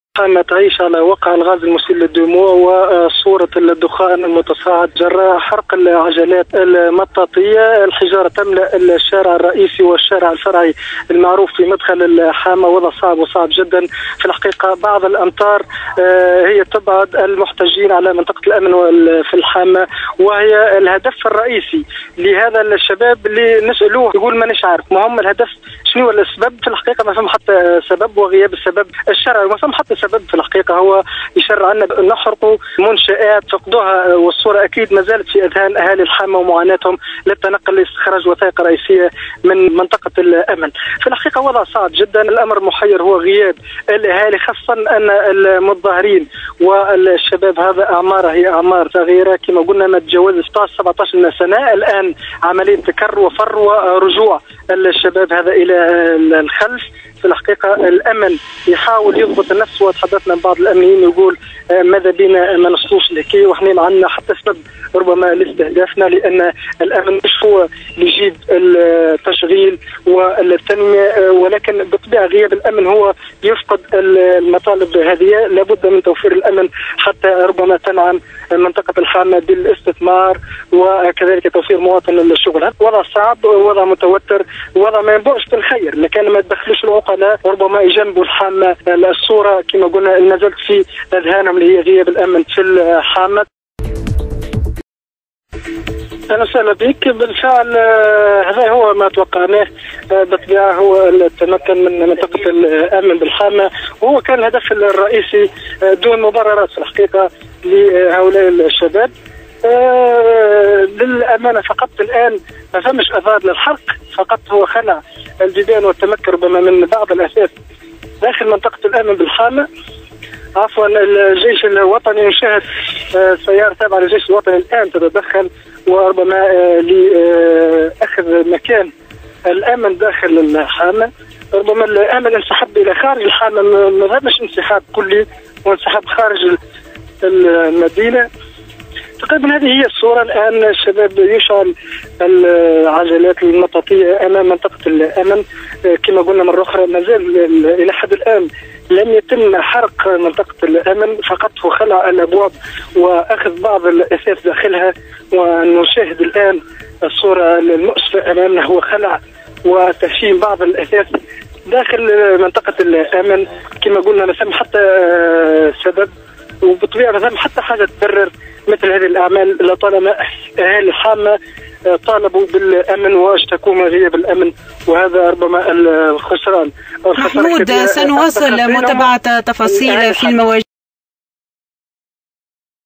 حسب مراسل أوازيس أف أم بالحامة محمود الدغباجي
خلع منطقة الامن الوطني بمعتمدية الحامة ولاية قابس بعد أن شهدت المعتمدية عشية اليوم احتجاجات واسعة واستعملت خلالها الوحدات الأمنيّة الغاز المسيل للدموع لتفريق المحتجّين.. الزميل محمود دغباجي ومزيد التفاصيل